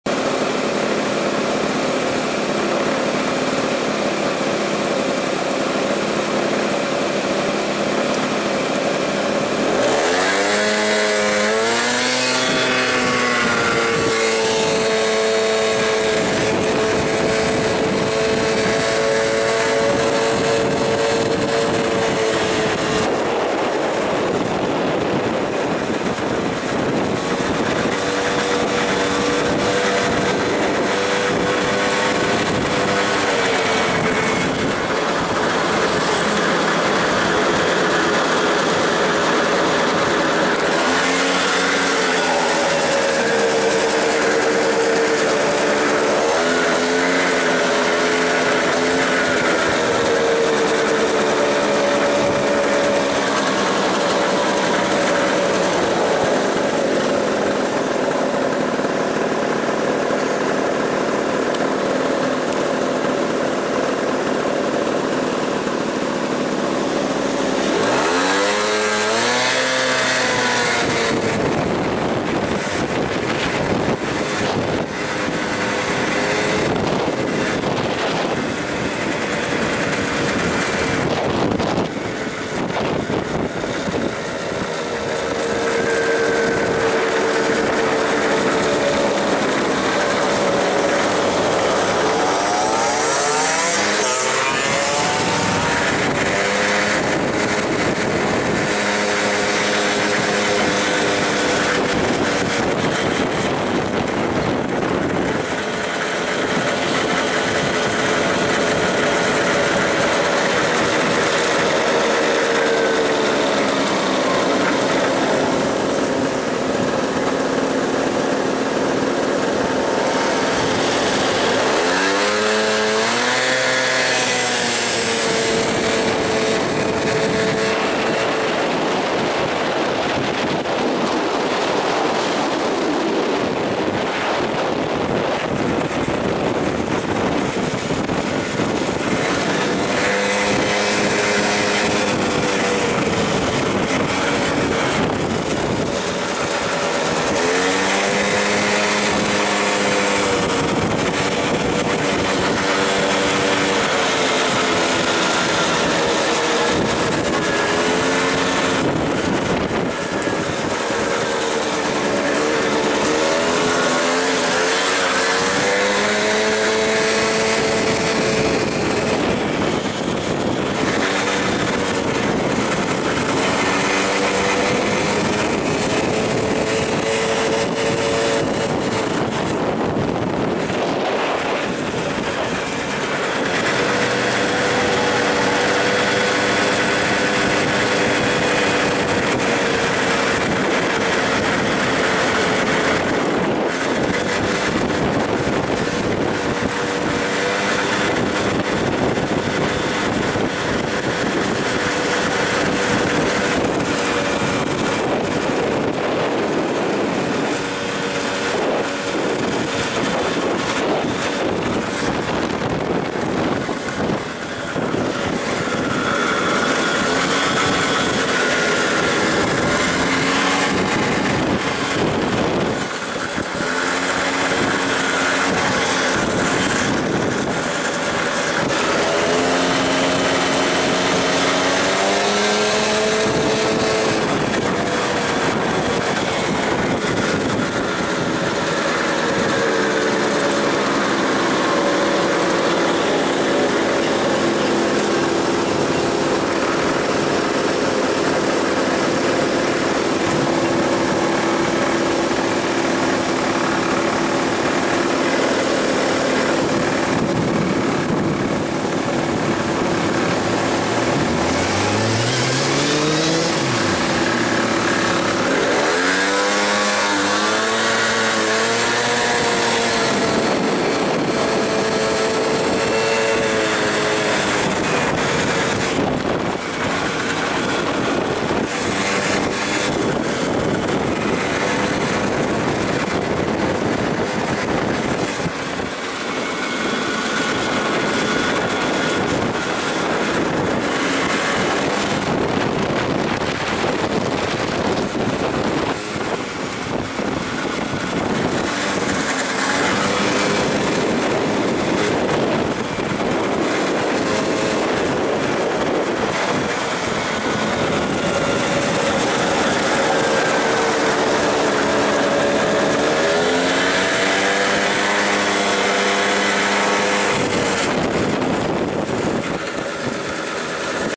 La promenade des Anglais en scooter Vespa LX50
Ballade en scooter sur la promenade des anglais a nice
Filmé avec sony PJ200